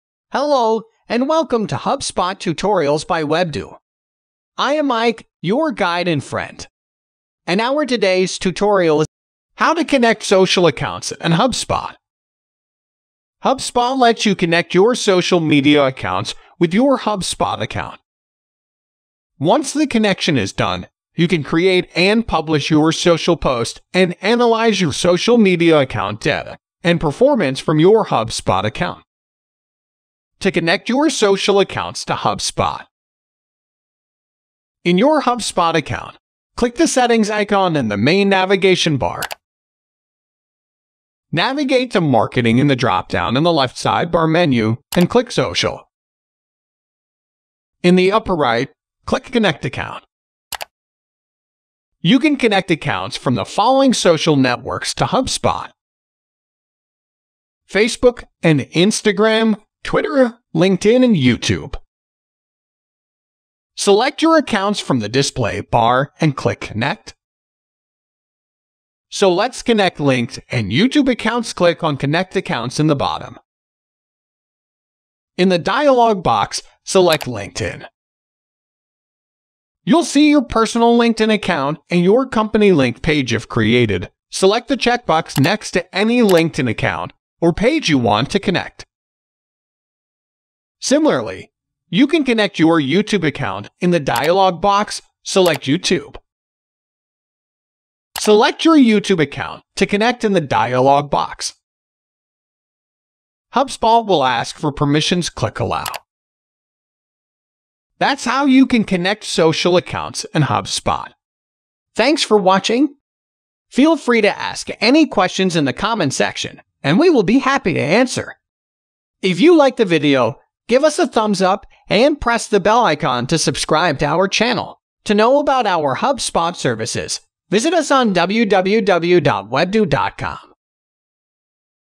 How to connect social accounts in HubSpot
In this HubSpot tutorial, we will walk you through how-to connect social accounts in HubSpot.

HubSpot lets you connect your social media accounts with your HubSpot account. Once the connection is done, you can create and publish your social posts and analyze your social media account data and performance from your HubSpot account.

Steps to Follow:

To connect your social accounts to HubSpot.

Step 1. In your HubSpot account, click the settings icon in the main navigation bar.

Step 2. Navigate to Marketing in the dropdown in the left sidebar menu and click Social.

Step 3. In the upper right, click Connect account.

Step 4. You can connect accounts from the following social networks to HubSpot.

Step 5. Facebook & Instagram, Twitter, LinkedIn, Youtube.

Step 6. Select your accounts from the display bar and click connect.

Step 7. Click on connect accounts at the bottom.

Step 8. In the dialog box, select LinkedIn.

Step 9. If created, you'll see your personal LinkedIn account and company linked page. Select the checkbox next to any LinkedIn account or page you want to connect.

Step 10. Similarly, you can connect your youtube account. In the dialog box, select YouTube.

Step 11. Select your youtube account to connect in the dialog box.

Step 12. Hubspot will ask for permissions.

Step 13. Click Allow.

That’s how you can connect social accounts in HubSpot.